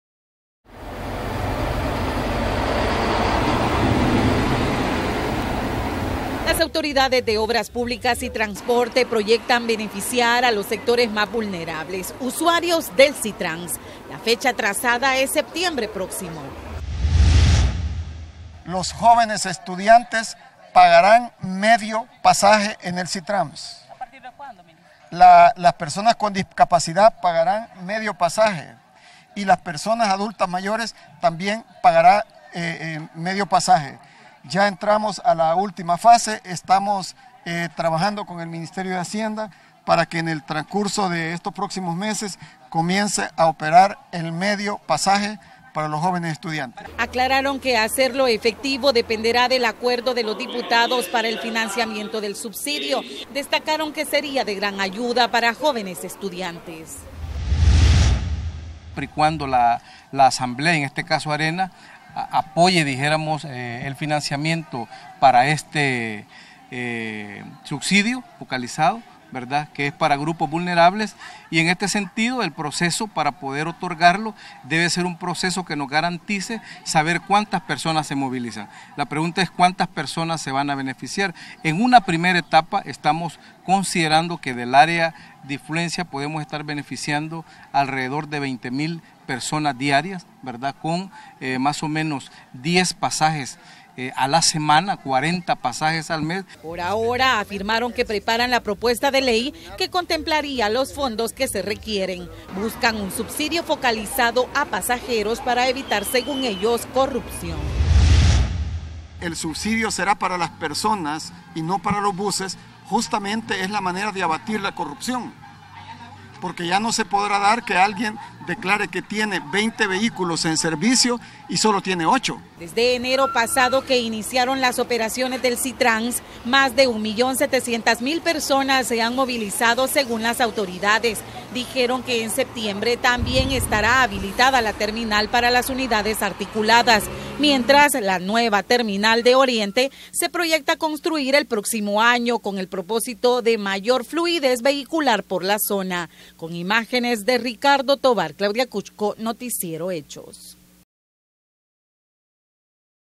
ANUNCIAN SUBSIDIO EN PASAJE DEL SITRAMSS
Las autoridades de obras públicas y transporte proyectan beneficiar a los sectores más vulnerables de los usuarios del Sitramss, la fecha trazada es septiembre próximo.